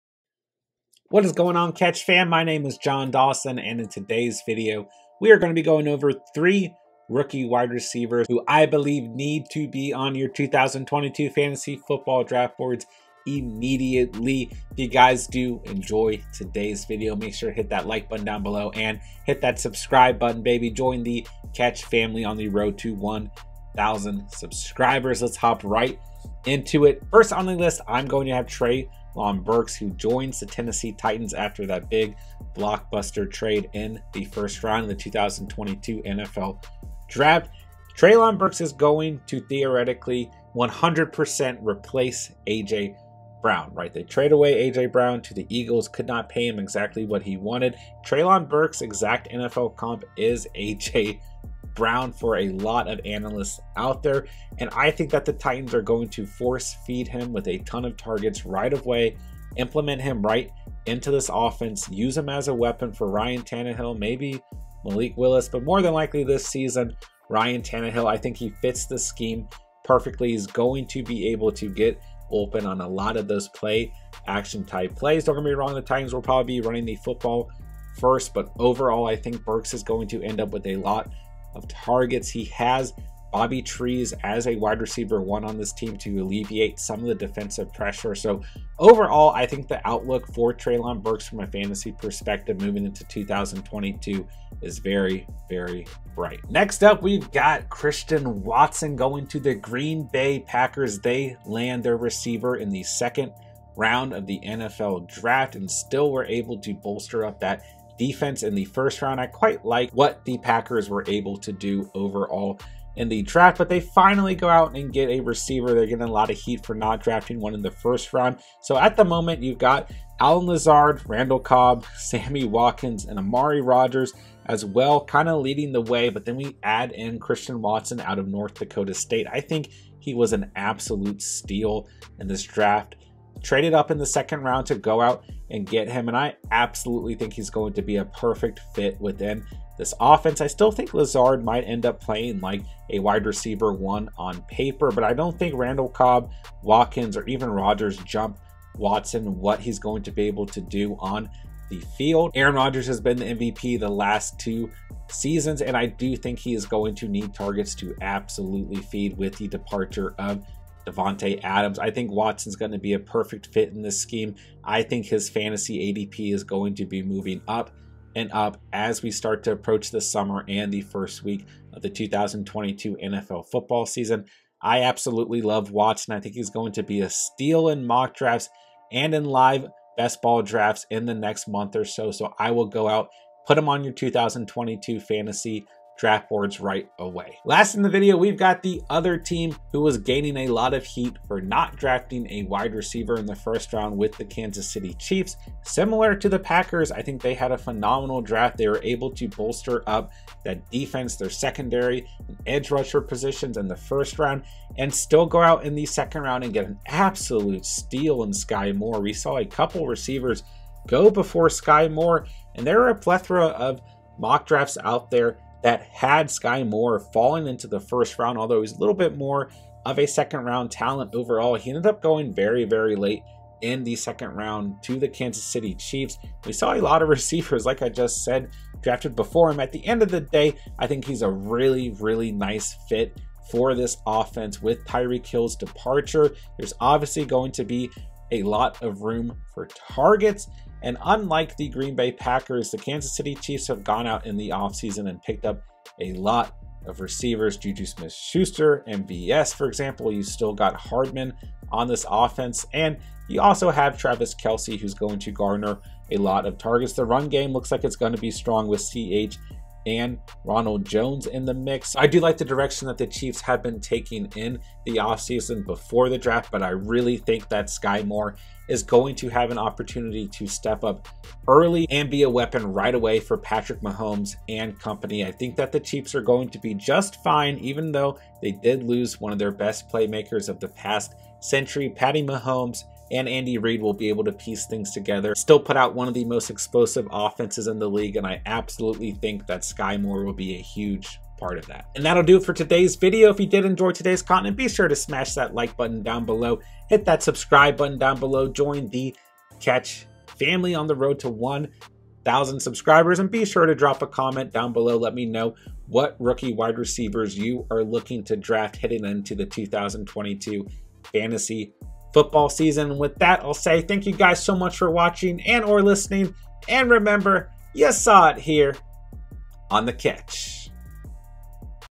3 Rookie Wide Receivers to Add to Your 2022 Draft Boards Right Away-2022 Fantasy Football
Welcome to The Catch!
In today's video we go over 3 rookie WRs to target in 2022 fantasy football drafts.

Play Underdog Fantasy Football with us!